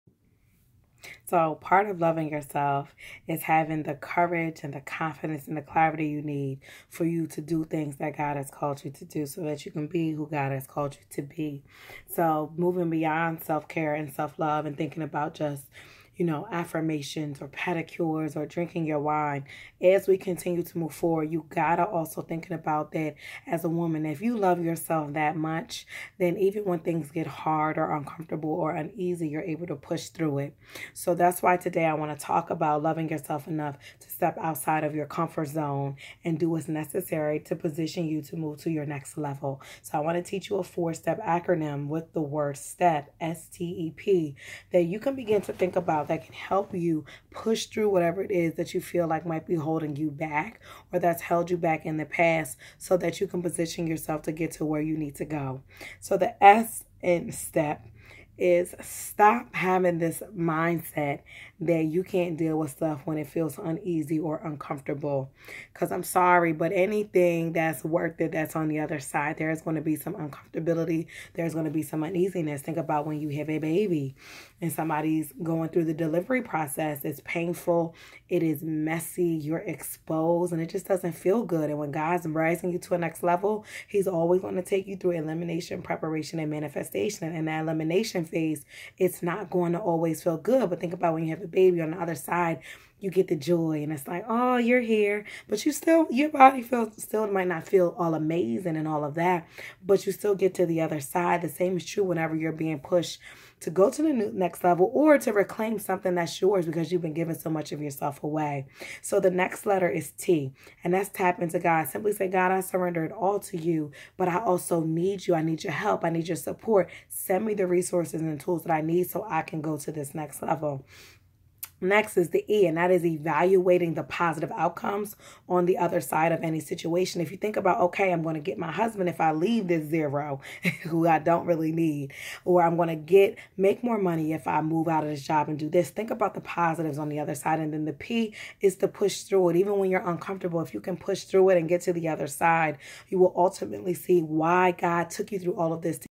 Loving Myself Enough to Step Out Of My Comfort Zone
Summer Self-Care Secrets Series: Video 1 Recap

For many of us we have been self-sabotaging for so long that we don’t even realize how bad we have gotten. While others need to learn to say “NO” more or stop being everything to everybody but yourself…

It’s time reclaim your time and re-establish order in your life in a way that benefits you and those around you because they get to see you at your BEST: in your overflow.

This series is not about bubble baths and drinking wine. No it’s about the very real DEEP inner work that true self-care takes: holding yourself accountable, putting yourself and your needs first, and working through toxic emotions or behaviors that need to be healed.

The days are as follows:

Day 1: Loving Myself Enough to Step Out Of My Comfort Zone

Day 2: Releasing Control and Taking Responsibility for The Woman God Call Me to Be

Day 3: No More Distractions & Excuses: How to Pay More Attention to Myself